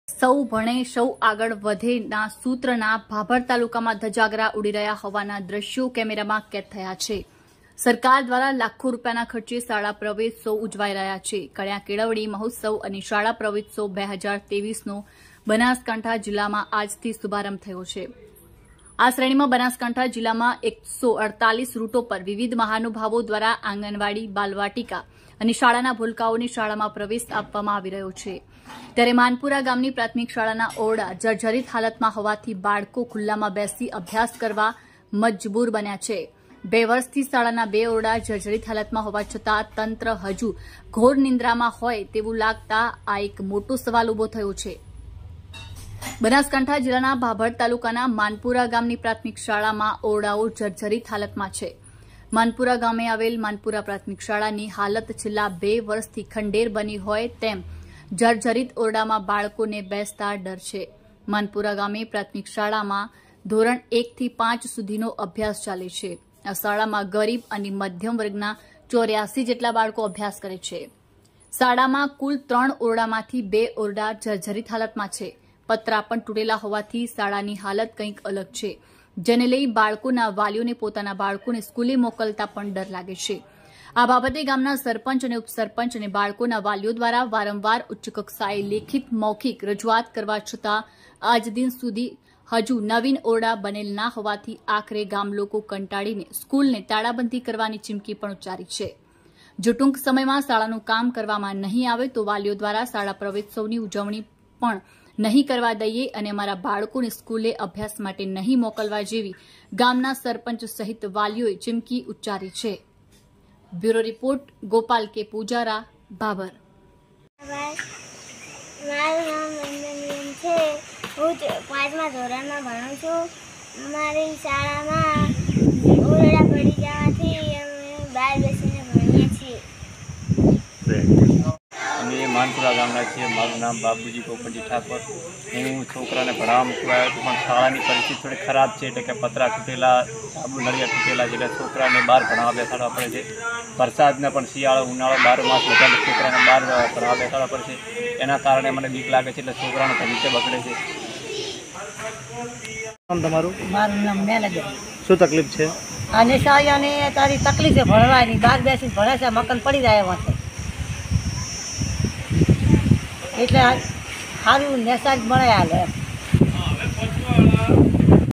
સૌ ભણે સૌ આગળ વધે સુત્રના ધજાગરા ઉડી રહ્યાં હોવાના દ્રષ્યો કેમેરામાં કેદ થયાં.....
@banasupdate સૌ ભણે સૌ આગળ વધે સુત્રના ધજાગરા ઉડી થયા ના દ્રષ્યો કેમેરામાં કેદ થયાં.